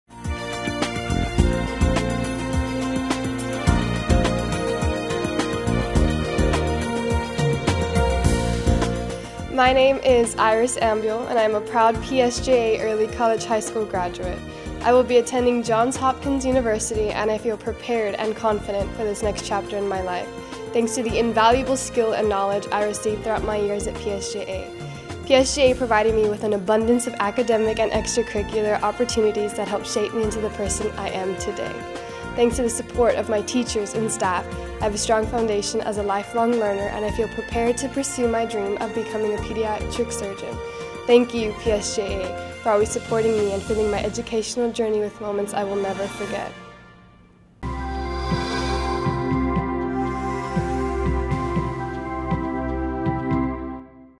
2021 PSJA Student Profile Iris Ambuehl
"PSJA provided me with an abundance of academic and extracurricular opportunities that helped shape me into the person I am today. Thanks to the support of my teachers and staff, I have a strong foundation as a life-long learner, and I feel prepared to pursue my dream of becoming a pediatric surgeon," Iris Ambuehl, PSJA Early College High School Class of 2021

While at PSJA ISD, Iris had the full high school experience getting to participate in rigorous academic programs while being actively involved in Mariachi, Cheer, and Student Council.

Through hard work and perseverance, Iris graduated with an Associate Degree in Biology through our Early College Program in partnership with South Texas College and will be attending one of our nation’s top institutions, Johns Hopkins University, with a full scholarship!